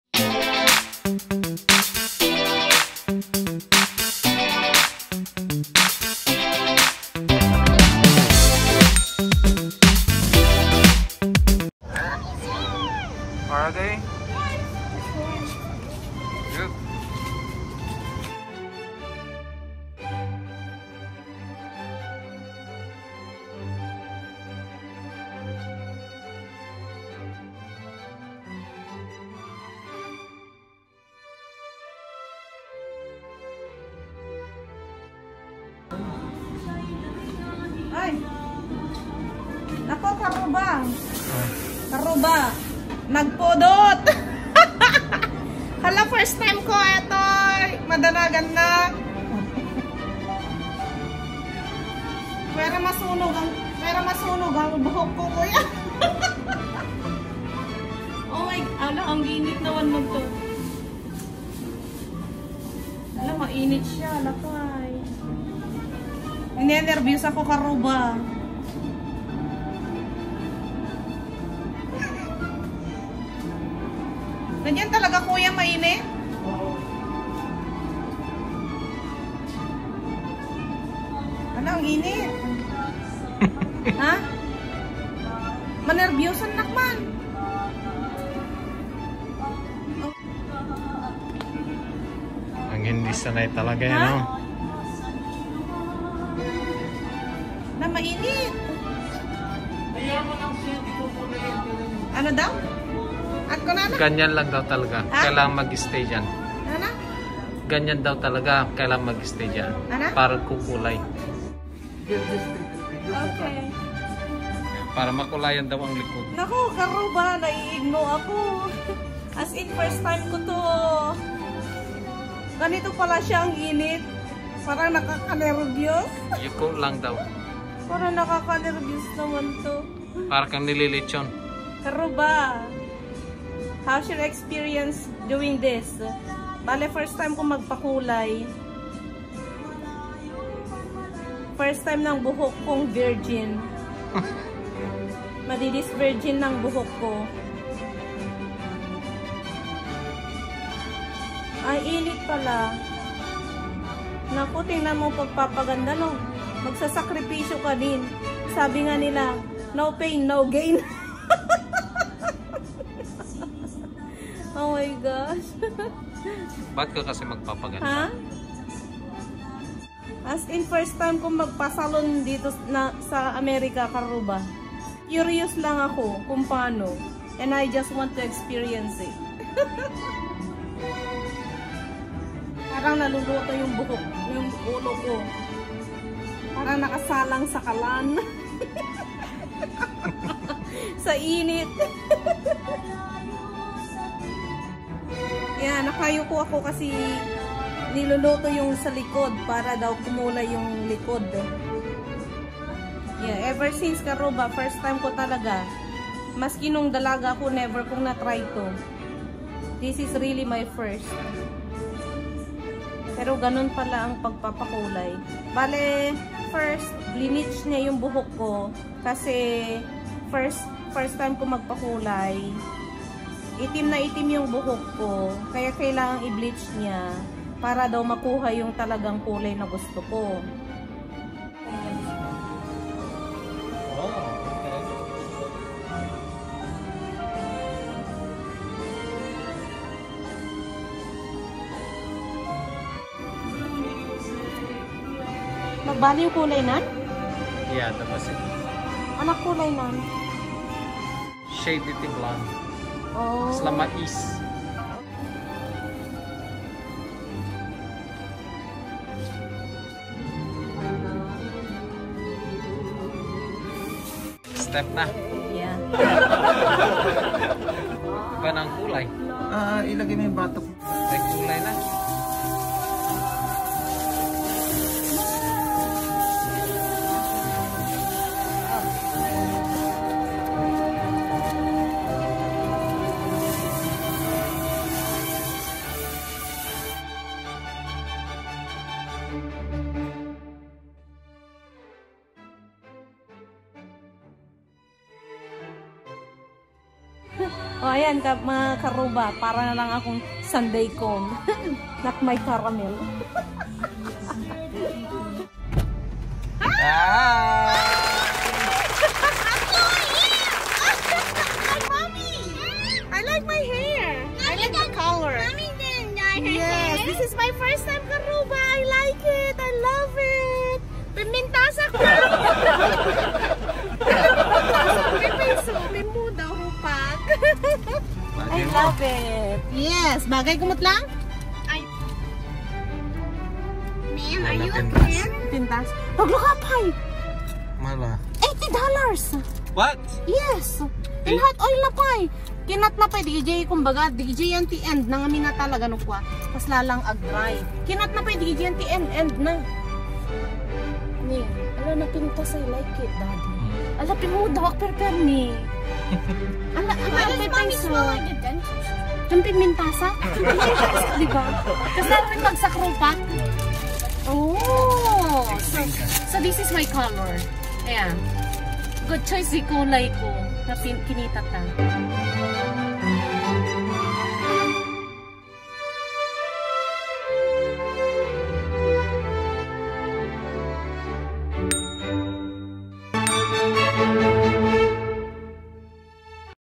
Hair Coloring/Bleaching and Hot Oil/NaLechon ang Brain Ko//Cabutajes TLP
My first time coloring and bleaching my hair!